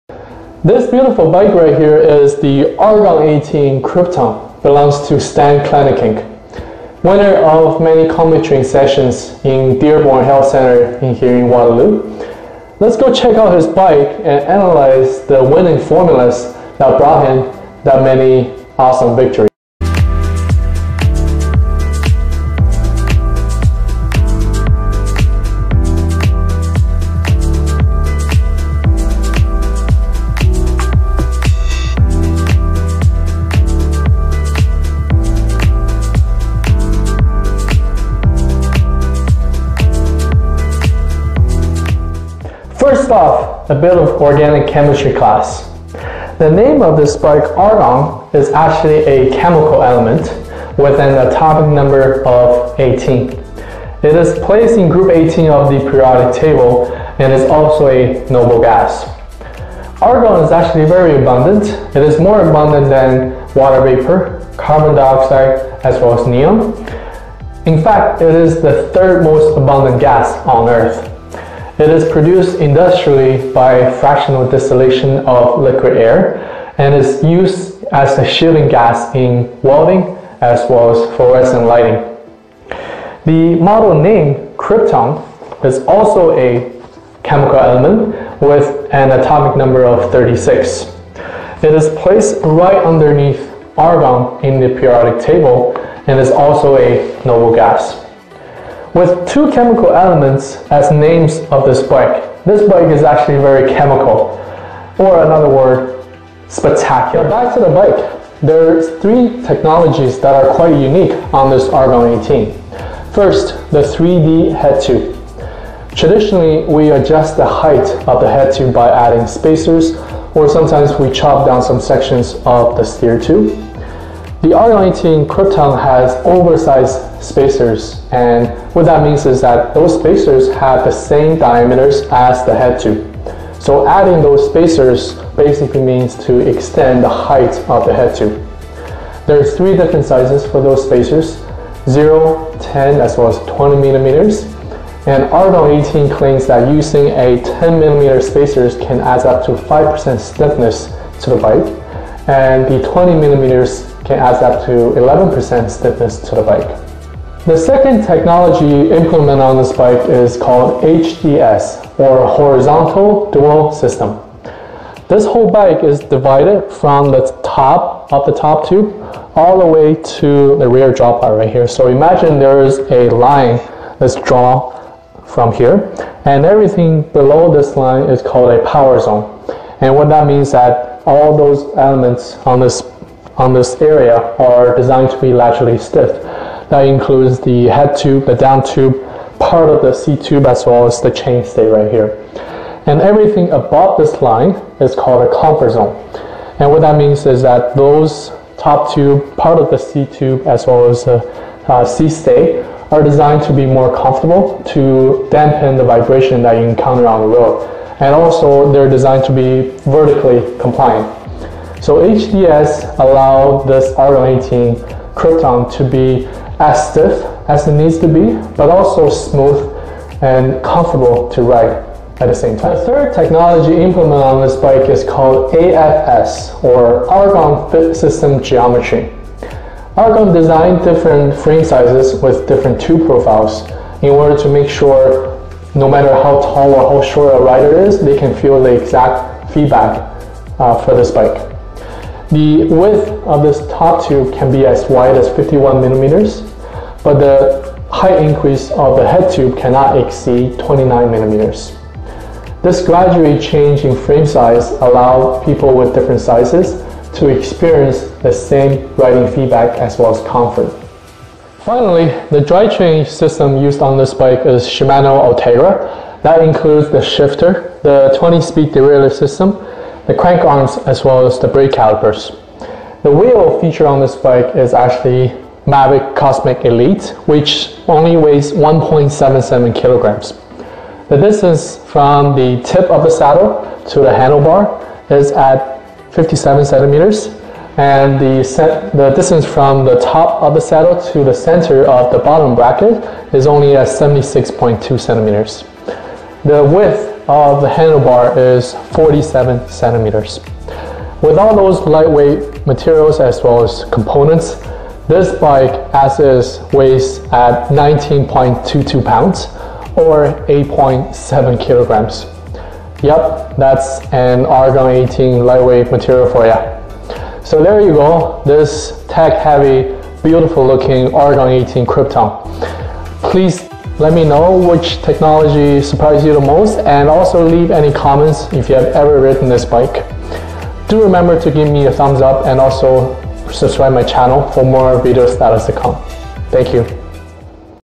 Episode #10 - Argon 18 Krypton
In this video i talked about the in house technology by Argon 18, the components and some scientific numbers.

Also check out my ad page for all the bikes i have in my inventory!